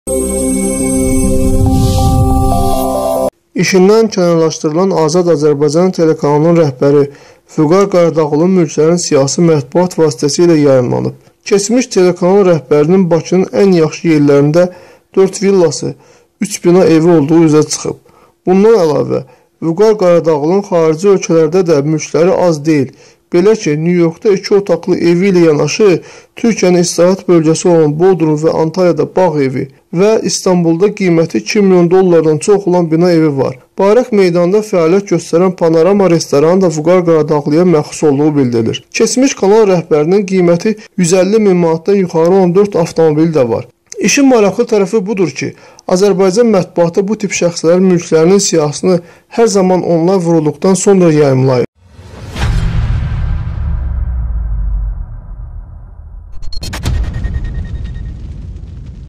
"2 milyona bir villa" - işindən kənarlaşdırılan ATV rəhbərinin mülklərinin siyahısı açıqlandı
İşindən kənarlaşdırılan Azad Azərbaycan telekanalının rəhbəri Vüqar Qaradağlının mülklərinin siyahısı mətbauat vasitəsi ilə yayaımlınıb. Keçmiş Telekanal rəhbərinin Bakının ən yaxşı yerlərində 4 villası 3 bina evi olduğu üzə çıxıb. Bundan Əlavə Vüqar Qaradağlının Xarici ölkələrdə də mülkləri az deyil. Belə ki, Nyu Yorokda 2 otaqlı evi ilə yanaşı Türkiyənin istirahət bölgəsi olan Bodrum və Antaliyada bağ evi və İstanbulda qiyməti 2 milyon dollardan çox olan bina evi var. Bayraq meydanında fəaliyyət göstərən  Panarama restoranınının da Vüqar Qaradağlıya məxsus olduğu bildirilir. Keçmiş kanal rəhbərinin qiyməti 150 min mantdan yuxarı olan 4 avtomobili də var. İş maraqlı tərəfi budue ki, Azərbaycan mətbuatı bu tip şəxslərin mülklərinin siyahısını hər zaman onlar vurulduqdan sonra yayınlayır.